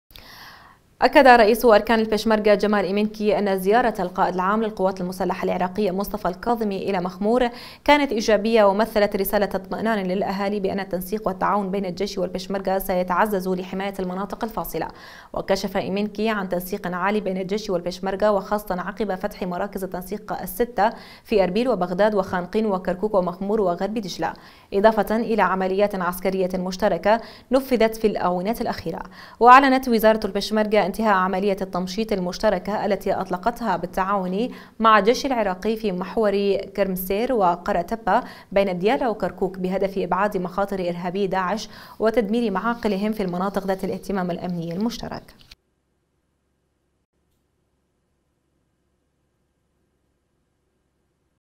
التنسيق بين الجيش و البيشمركة مرحلة متقدمة لنهاية داعش
أكد رئيس أركان البيشمركة جمال ايمنكي ان زيارة القائد العام للقوات المسلحة العراقية مصطفى الكاظمي، الى مخمور كانت ايجابية ومثلت رسالة اطمئنان للأهالي بان التنسيق والتعاون بين الجيش والبيشمركة سيتعزز لحماية المناطق الفاصلة؛ وكشف ايمنكي عن تنسيق عال بين الجيش والبيشمركة وخاصة عقب فتح مراكز التنسيق الستة في اربيل وبغداد وخانقين وكركوك ومخمور وغرب دجلة، اضافة الى عمليات عسكرية مشتركة نفذت في الاونة الاخيرة، وأعلنت وزارة البيشمركة انتهاء عملية التمشيط المشتركة التي أطلقتها بالتعاون مع الجيش العراقي في محور كرمسير وقرة تبة بين ديالى و كركوك بهدف إبعاد مخاطر إرهابيي داعش وتدمير معاقلهم، في المناطق ذات الاهتمام الأمني المشترك.